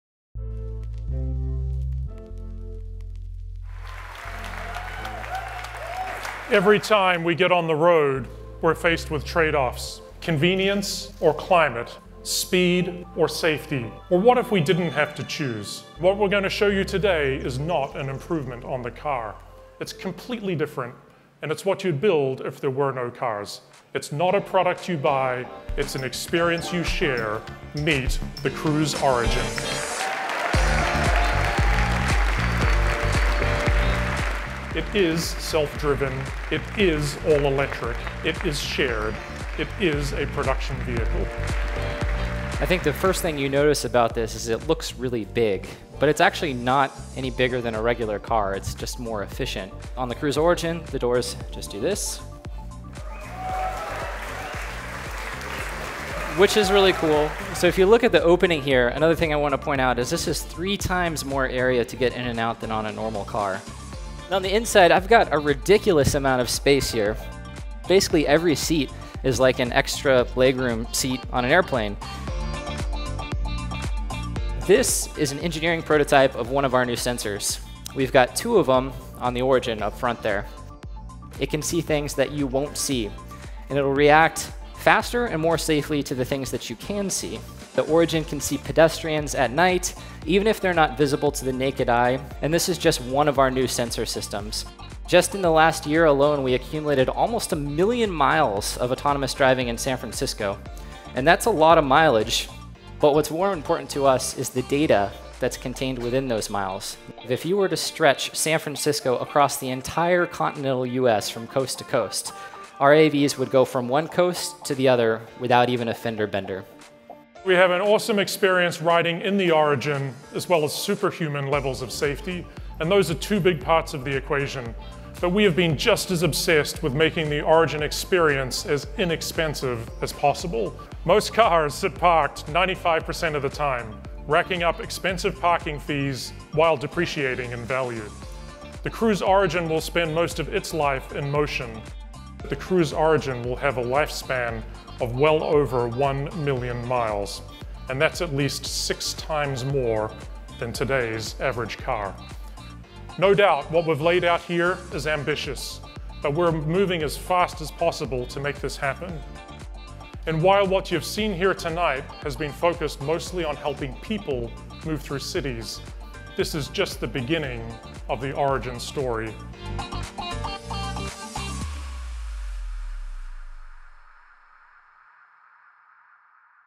GM 크루즈 레벨 5 자율주행차 오리진(the Cruise Origin) 발표 영상
2020년 1월 22일,  GM 크루즈는 자율주행 레벌 5 단계가 가능한 로보택시 '오리진(the Cruise Origin)'을 발표했습니다.